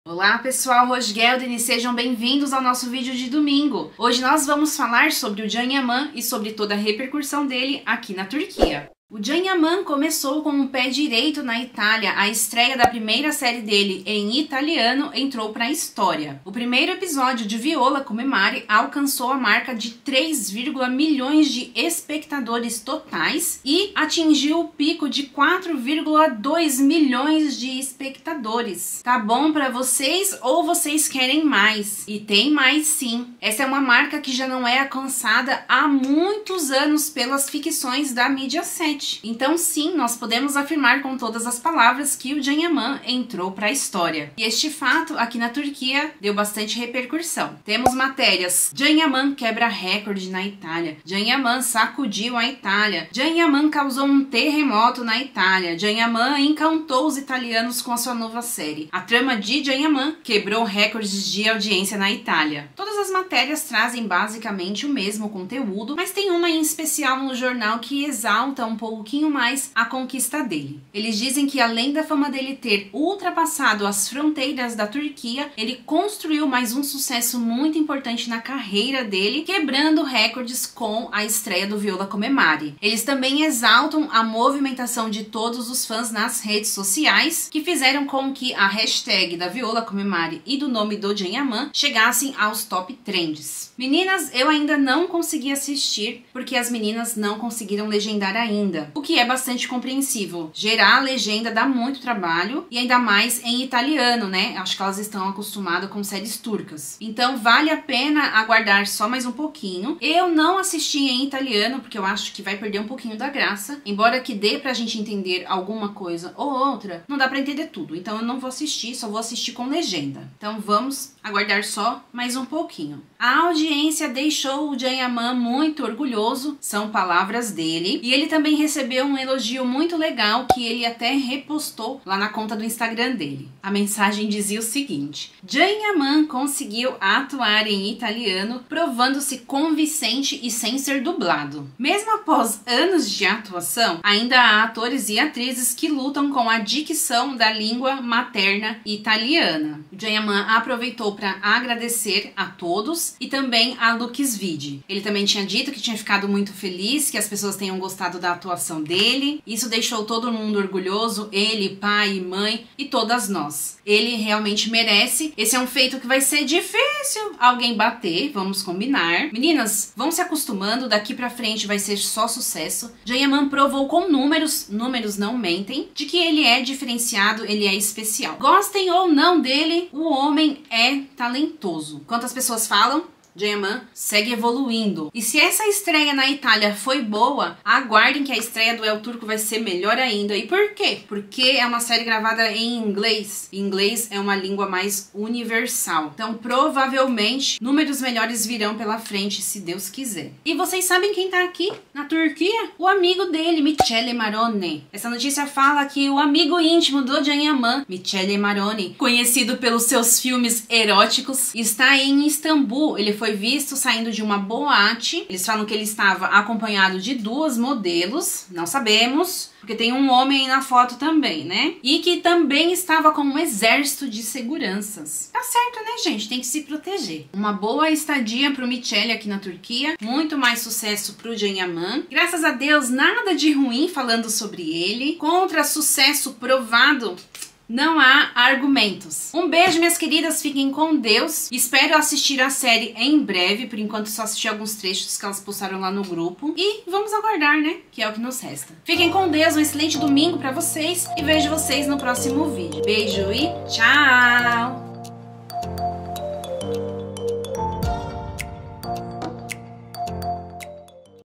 Repercussão na Turquia. Can Yaman faz história com primeiro episodio de Viola Comi il Mare
O primeiro episodio de Vila Comi El Mare com o ator Can Yaman e a Francesca Chillemi foi um sucesso na Itália, nesse video falo sobre a repercussão do sucesso de Can Yaman na Turquia com a sua nova série Viola Comi il mare.

Olá, eu sou a Denise e o meu Instagram é o @vivendonaturquia